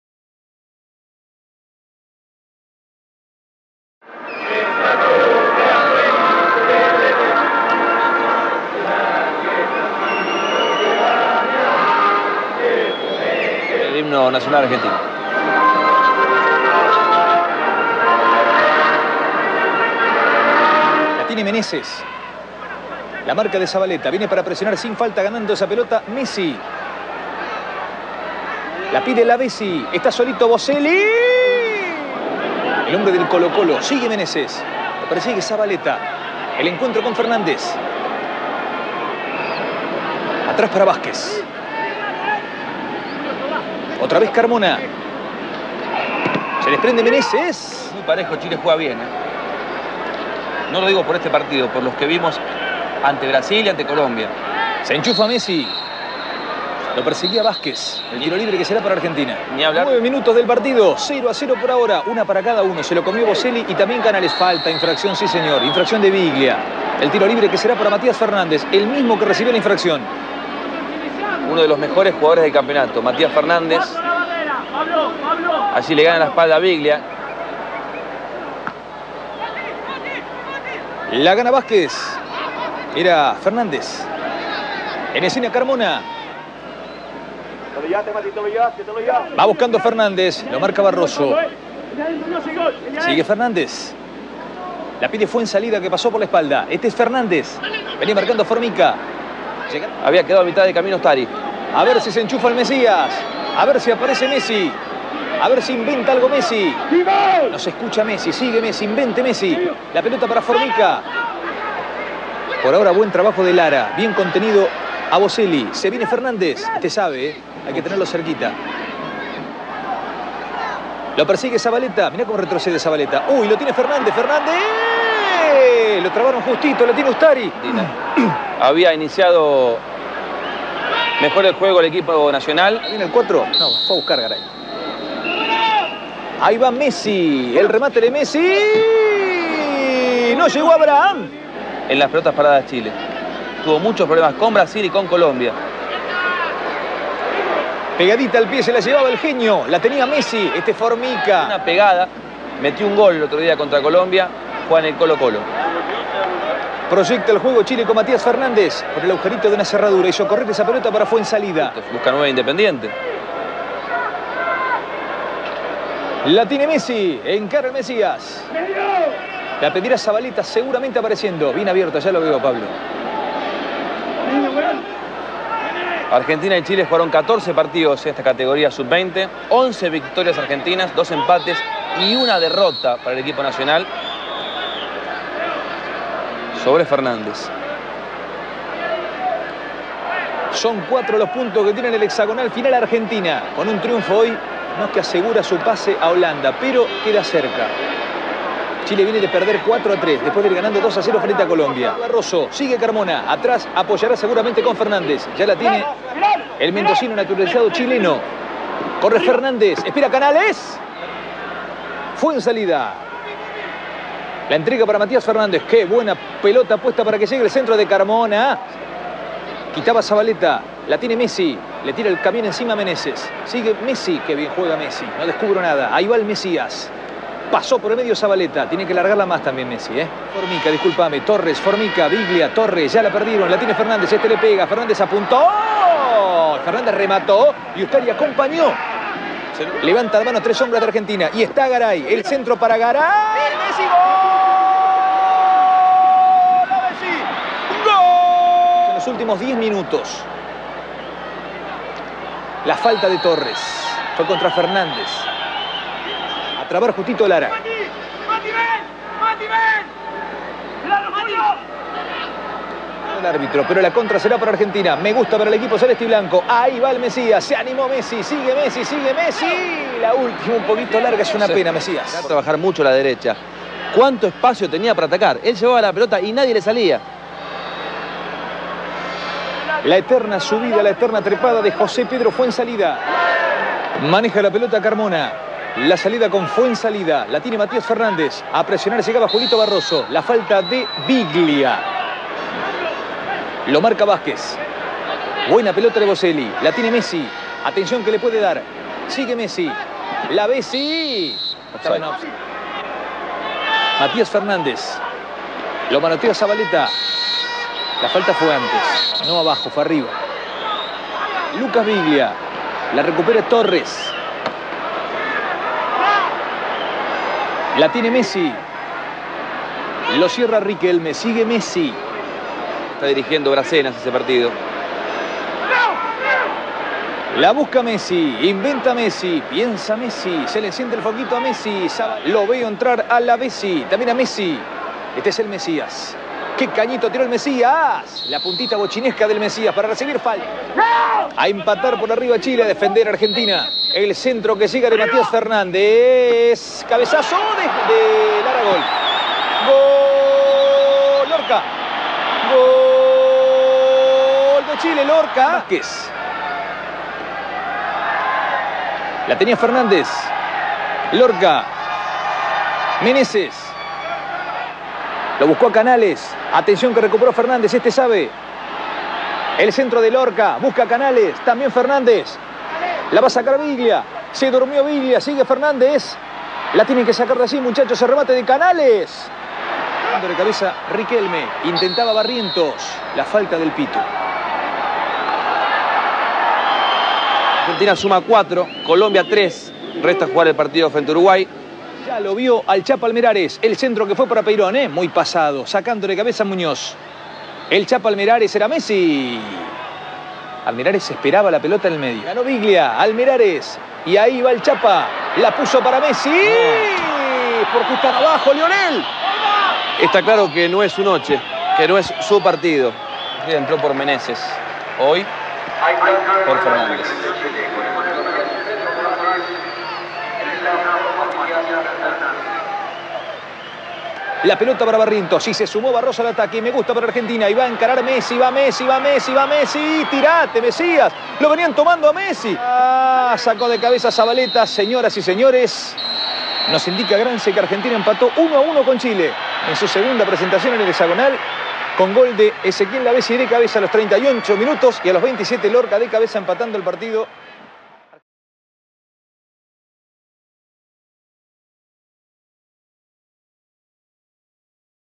Cuando se enfrentaron MESSI y MATI FERNÁNDEZ - Sudamericano U-20 / 2005.
Seguimiento a Matías Fernández y a Lionel Messi, en el partido entre las selecciones sub 20 de Argentina y Chile, válido por el sudamericano de Colombia 2005.
Ambos eran las figuras de sus equipos y del campeonato y en este partido hicieron todo lo posible por entregarle el triunfo a su selección y así  acercarse cada vez más al mundial de Holanda, que se disputaría el mismo año.